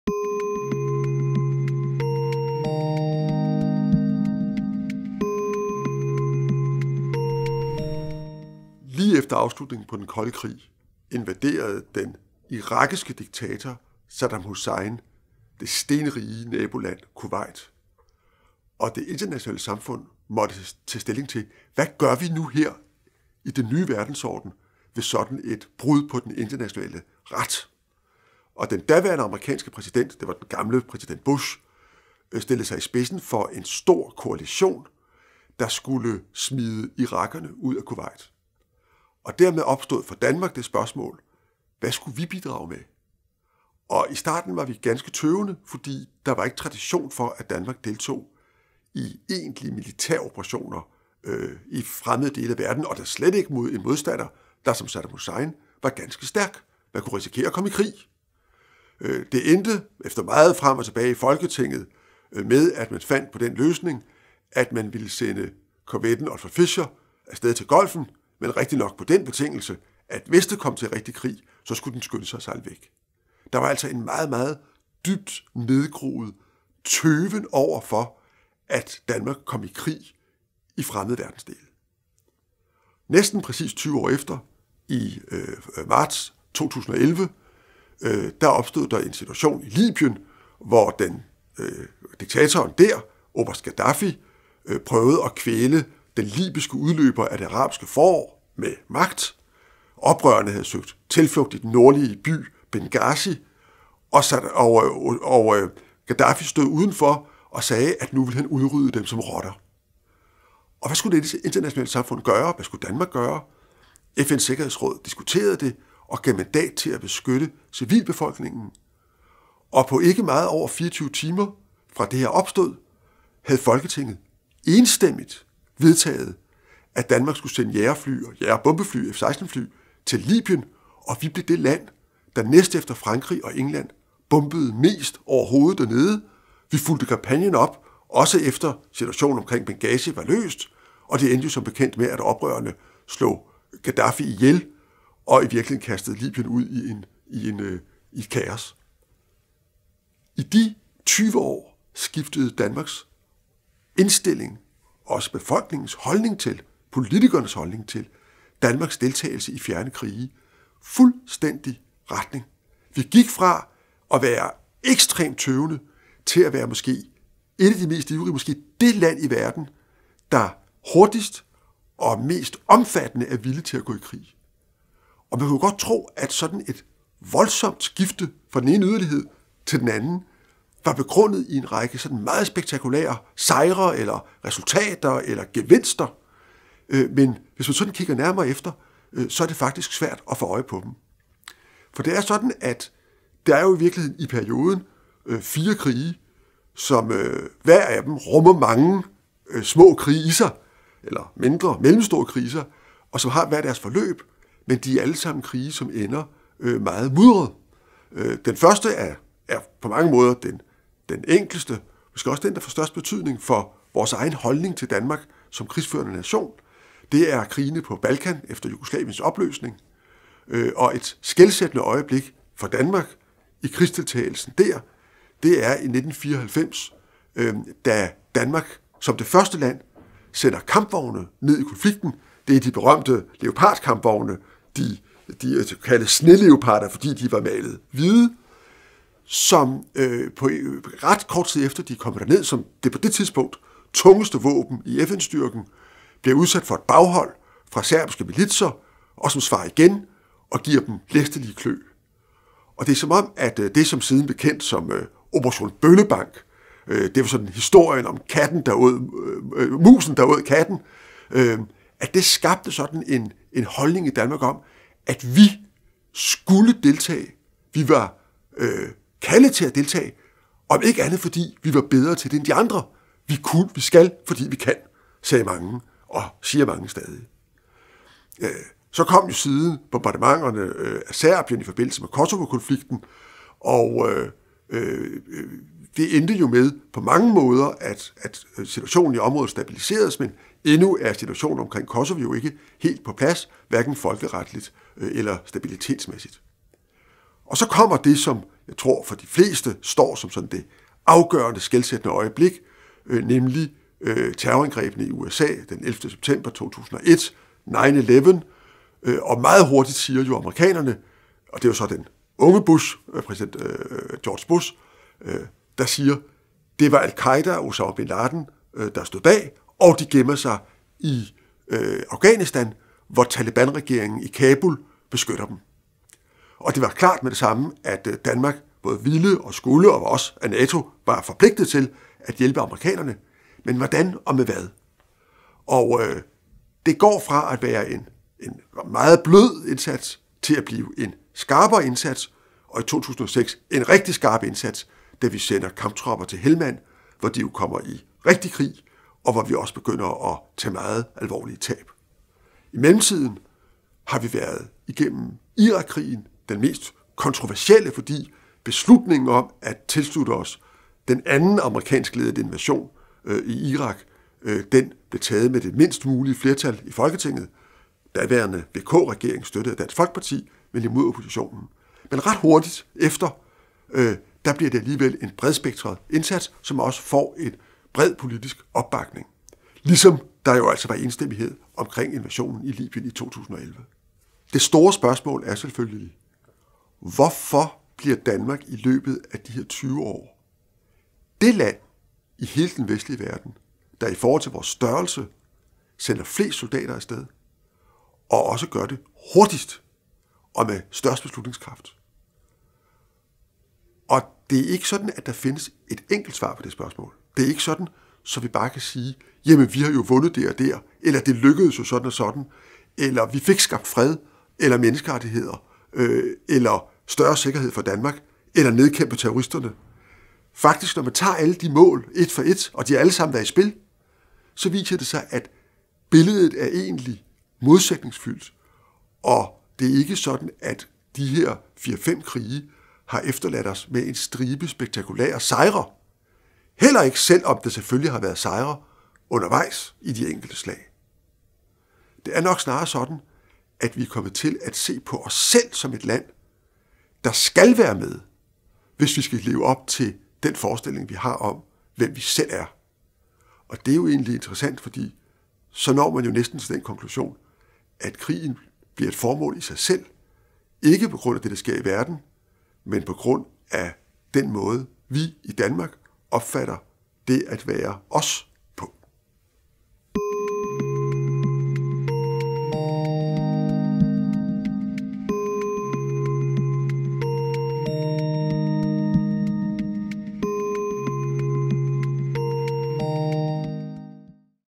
Danmark i krig 1991-2011
Da Irak under ledelse af Saddam Hussein (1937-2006) i august 1990 invaderede det olierige naboland Kuwait, blev det i FN's Sikkerhedsråd besluttet, at man foruden økonomiske sanktioner måtte anvende militær magt til at tvinge Irak tilbage. Efter 125 år uden deltagelse i skarpe krigsindsatser diskuterede man intenst i Danmark, hvorvidt og hvor meget man skulle bidrage til opgøret med Saddam Hussein. Efter lang tids tøven vedtog man i 1991 at sende krigsskibet Olfert Fischer til Den Persiske Golf og dermed tilslutte sig den første Golfkrig (januar-februar 1991).

Mellem 1991 og 2011 deltog Danmark i krigene på Balkan (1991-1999), i Afghanistan (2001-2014) og i Irak (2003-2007). Og i de første måneder af 2011 bredte Det Arabiske Forår sig til Libyen og statslederen Muammar al-Gaddafi (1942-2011) forsøgte med magt at nedslå oprørerne. Denne gang var Folketinget kun lidt over et døgn om enstemmigt at beslutte, at Danmark skulle sende F16 fly til Libyen og bidrage med omfattende bombardementer i en større luftstyrke med Frankrig og Storbritannien i spidsen. På 20 år var holdningen til krig forandret, så Danmark i højere grad end andre lande bidrog til internationale militære operationer. En holdningsændring til krigsdeltagelse havde dermed fundet sted.

Bo Lidegaard, historiker, dr.phil., tidligere topembedsmand og chefredaktør, giver sit bud på, hvor holdningen til dansk krigsdeltagelse ændrede sig så meget fra 1991 til 2011.